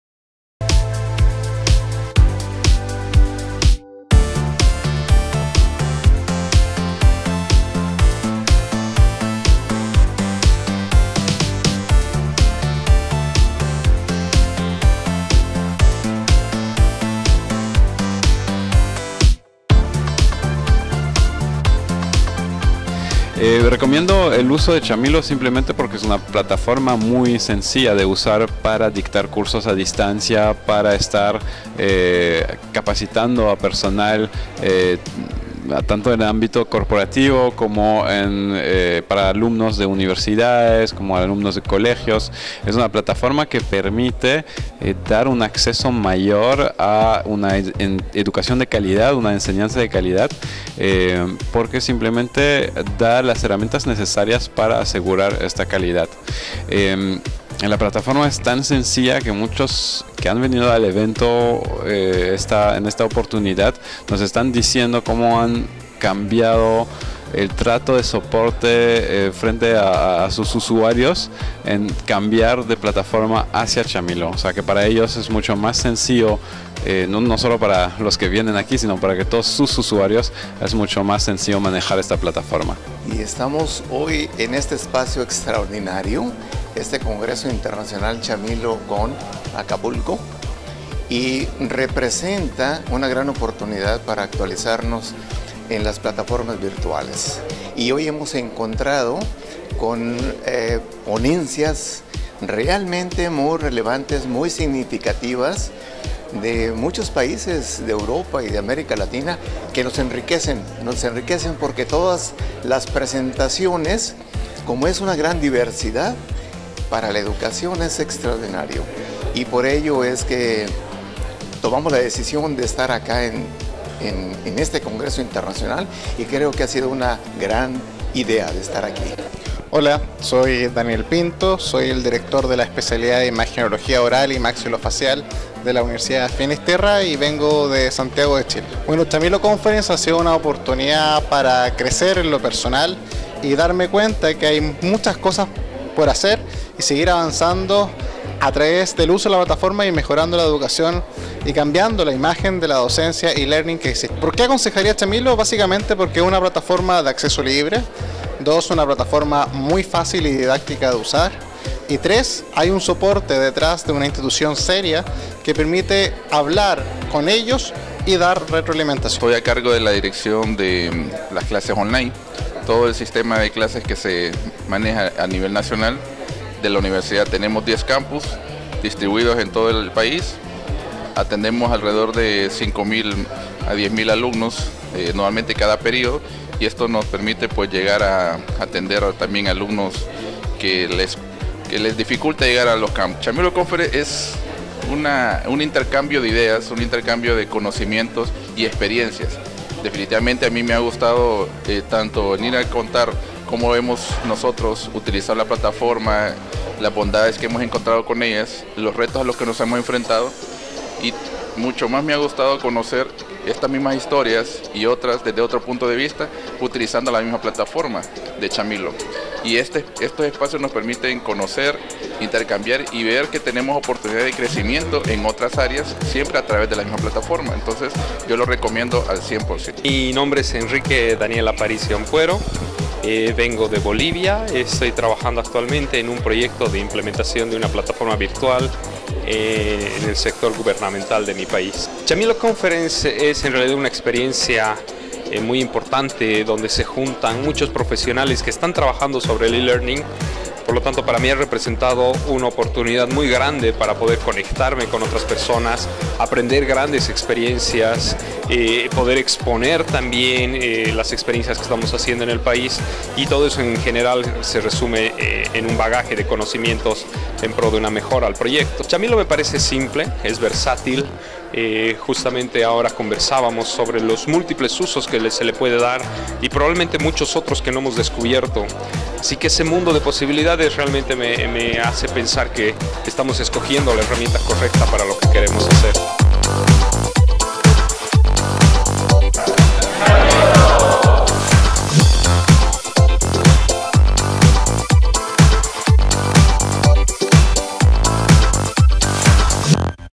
Entrevistas Chamilo Conference 2018
IV Congreso Internacional de Plataforma E-learning Chamilo LMS
Lugar: Acapulco - México, los días 07 y 08 Noviembre 2018